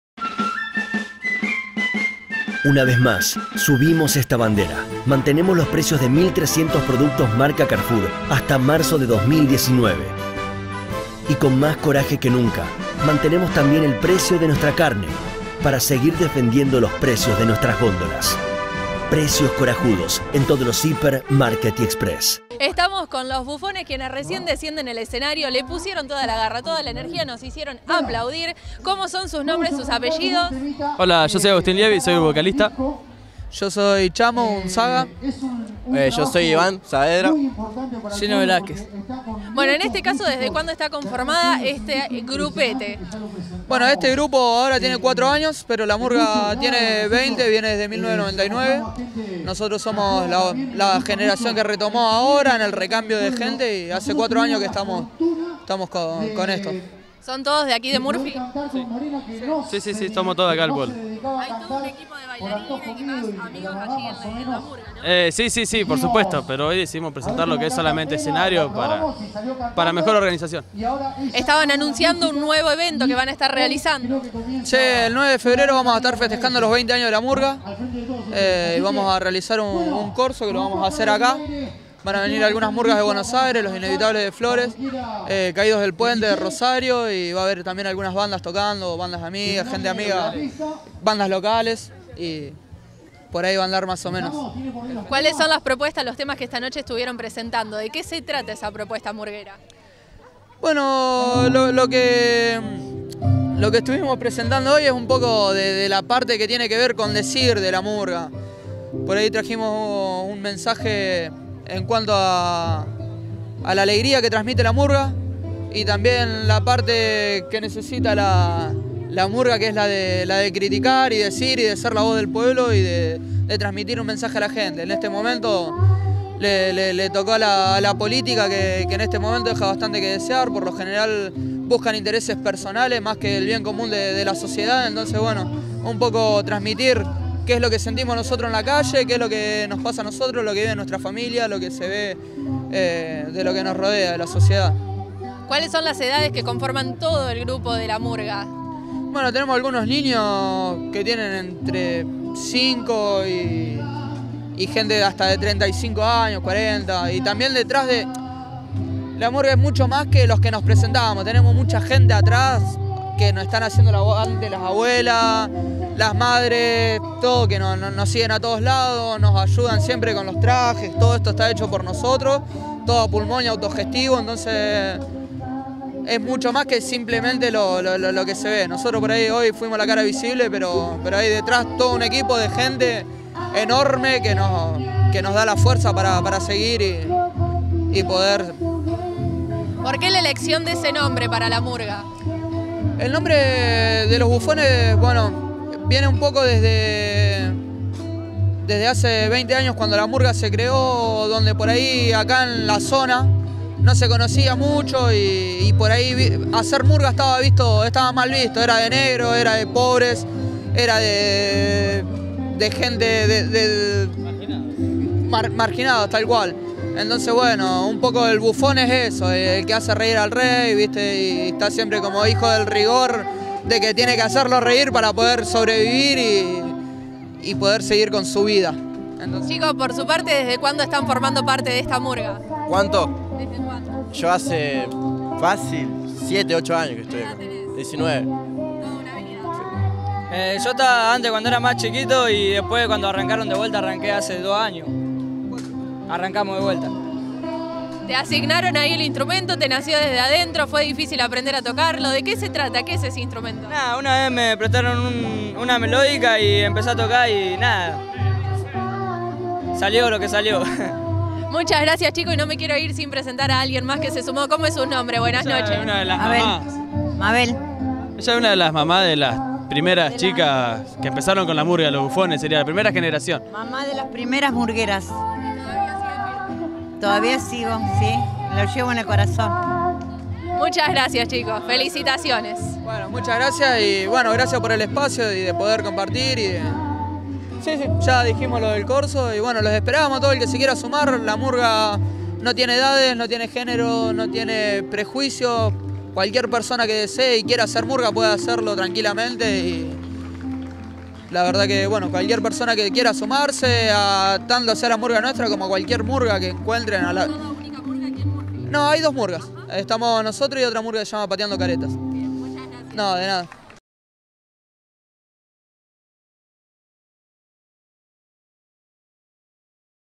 108 AÑOS LOCALIDAD DE MURPHY- Multimedio Grupo Straffela
Nota a Murga "Los Bufones" en el festejo de los 108 años de la localidad de Murphy